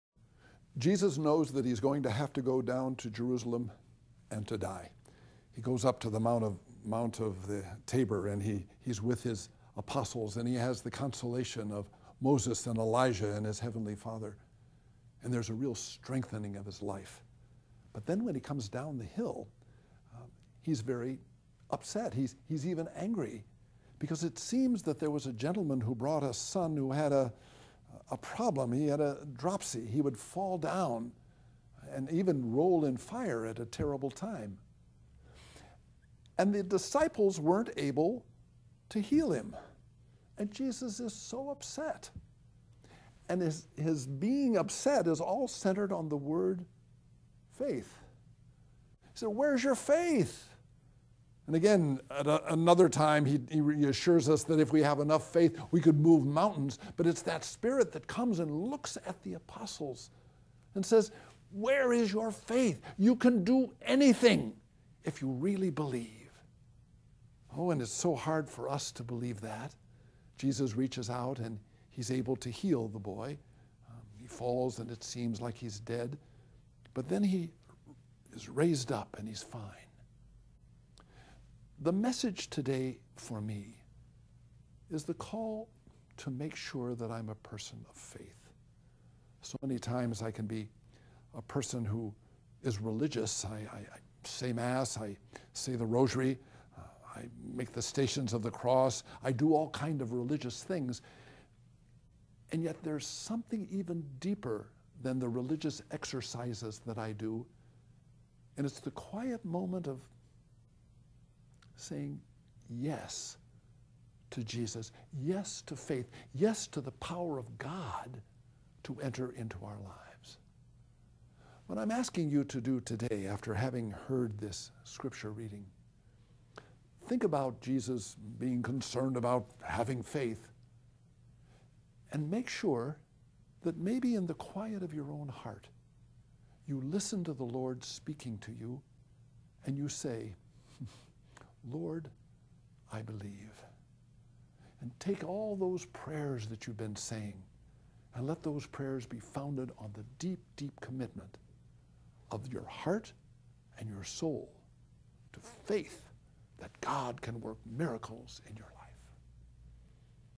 Do I really believe?
Sometimes people who claim to believe in God do a lot of religious actions: go to churc h, read the Bible and have various devotions. Today we must ask ourselves if our faith is real and not just a bunch of religious activities. We need to take a quiet moment and acknowledge our faith.